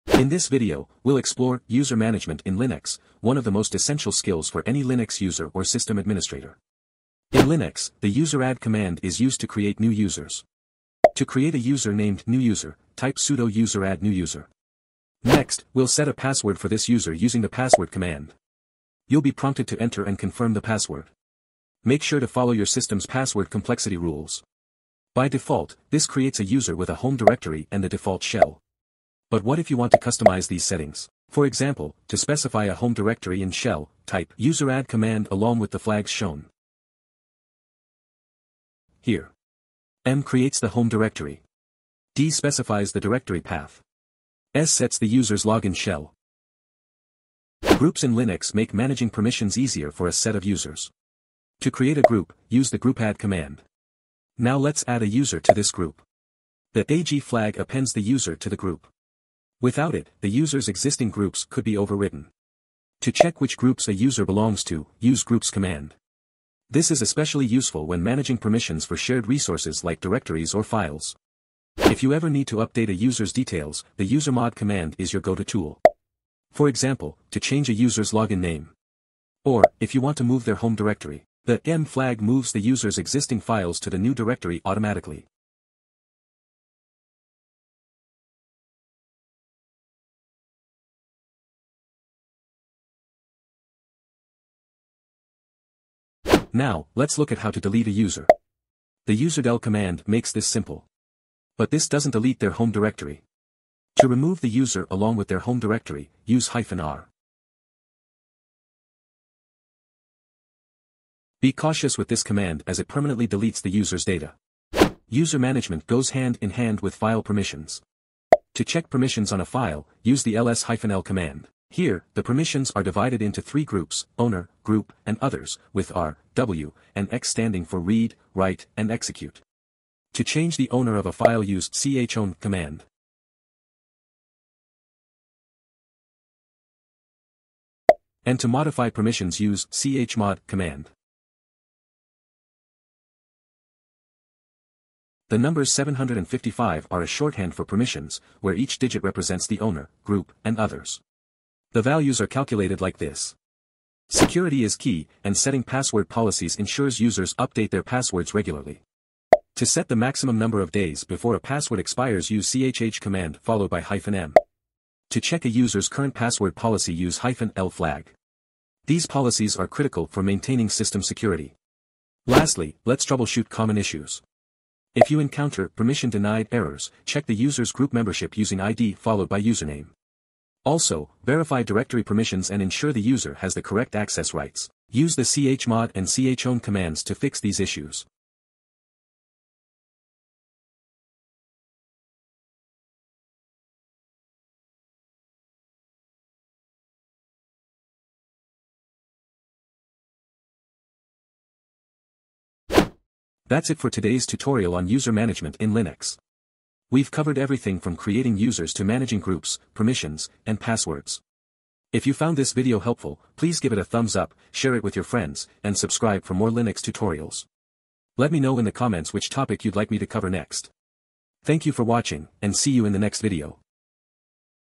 User Management in Linux | Complete Guide to Users, Groups, and Permissions
Master User Management in Linux with this step-by-step tutorial! Learn how to create and delete users, manage groups, modify user details, and set file permissions. This video covers essential commands like `useradd`, `usermod`, `userdel`, `groupadd`, `chmod`, `chown`, and more. Perfect for Linux beginners and system administrators alike. Don't miss out on tips for troubleshooting and enhancing system security with password policies. Watch now and level up your Linux skills!